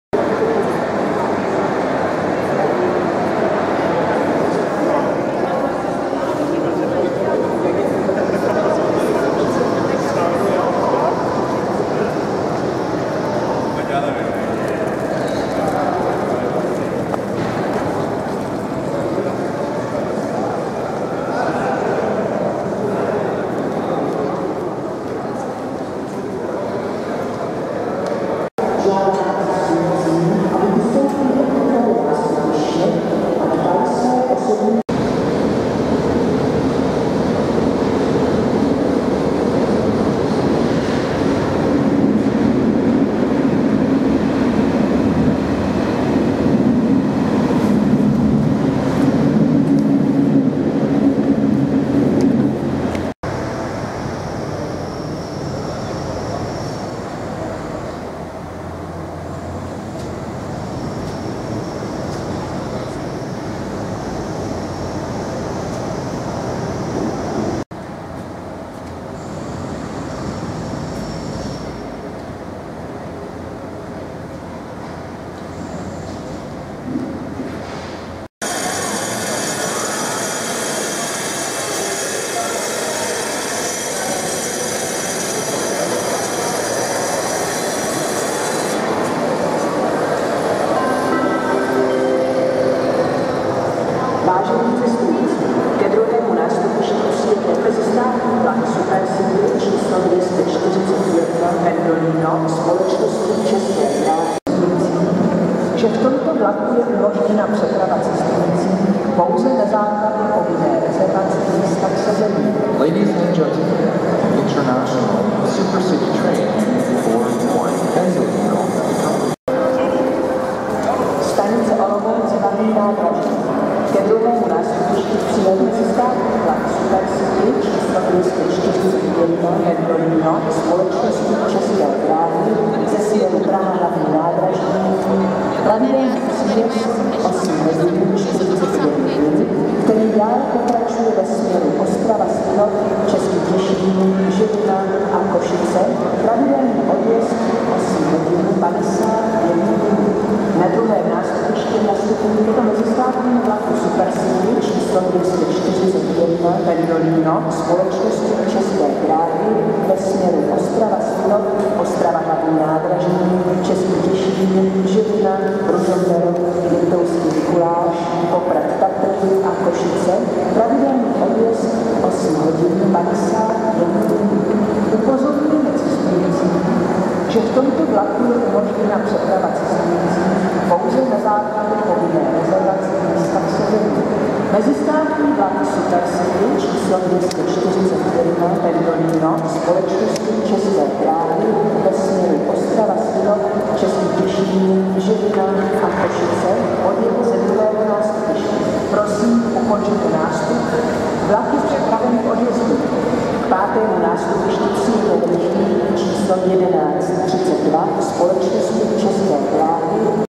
Železniční stanice Olomouc revitalizace | eskalátory 10.4.2015
V železniční stanici Olomouc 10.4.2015 provozní eskalátory (od 8.4.2015) ve vstupní hale (ještě se dokončuje okolí). Jejich provoz prověří cestující. Již v "nedopravní špičku" problémy ..., a kde bude únikový východ? Zajímavé architektonické řešení ...